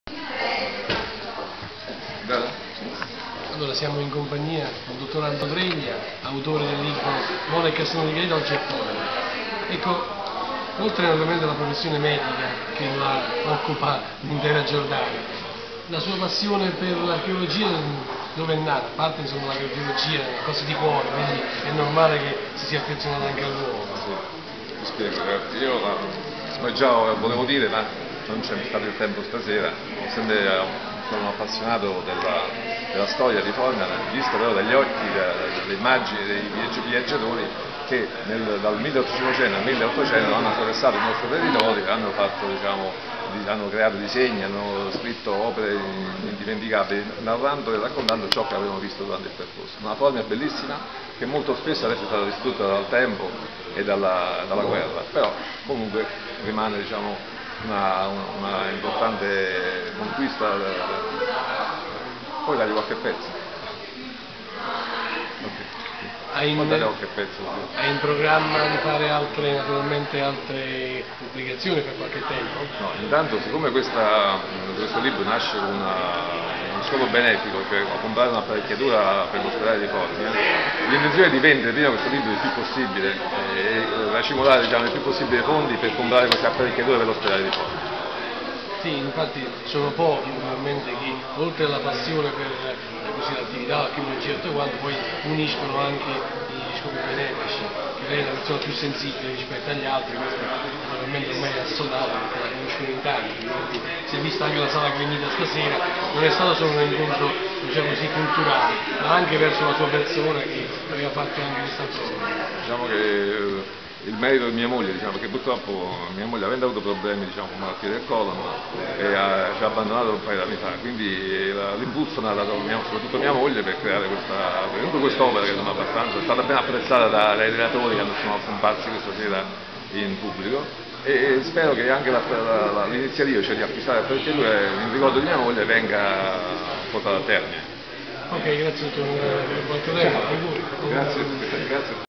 Dott. Aldo Treglia
Video-Intervista al Cardiologo dell'Ospedale di FORMIA(LT) in occasione della PRESENTAZIONE del Libro 'Mola e Castellone di Gaeta,Oggi Formia. Memorie e Immagini dei viaggiatori del Gran Tour' del Cardiologo Aldo Treglia.
Video di Federico Galterio e Intervista di Alessio Pimpinella.